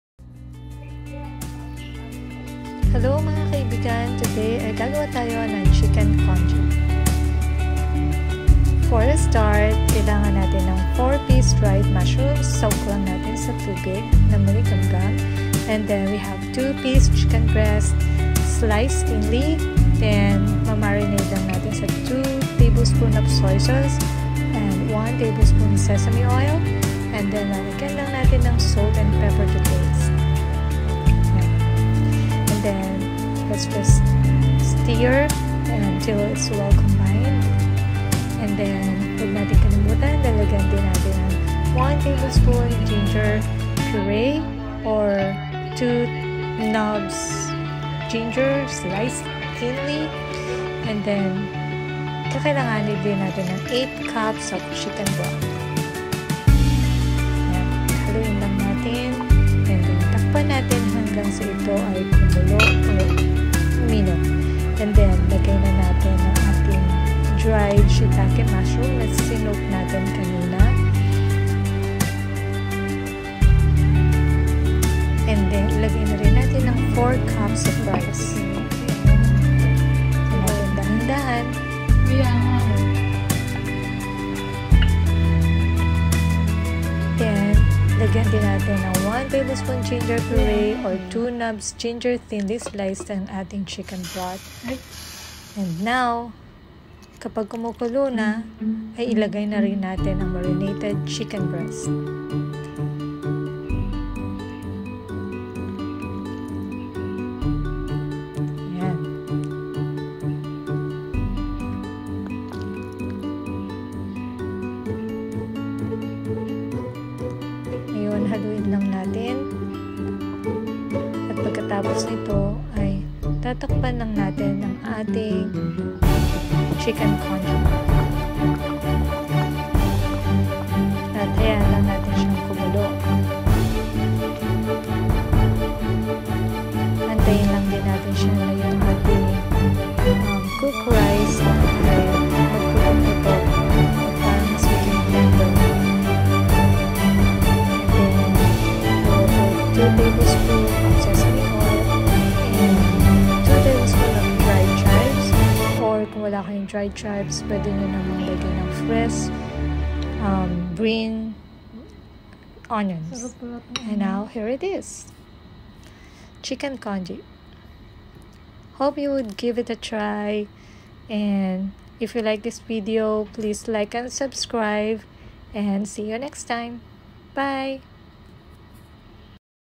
NAPAKADALI PALANG MAGLUTO NG CHINESE CHICKEN CONGEE! | BEST EASY AND DELICIOUS CHICKEN CONGEE
This chicken congee is really very tasty. It is only made of few ingredients and it is so easy to make. Very satisfying especially in a cold weather. Hope you give it a try.

Ingredients:
2 pcs chicken breast, sliced thinly
3-4 pcs dried shitake mushrooms, sliced thinly or minced
3-4 cups cooked rice
8-10 cups chicken broth
1 tbsp grated ginger or 2 knobs julienned
1 tbsp sesame oil
2 tbsp dried chives or fresh green onion

Chicken Marinade:
1 tbsp grated ginger or 2 knobs julienned
1 tbsp sesame oil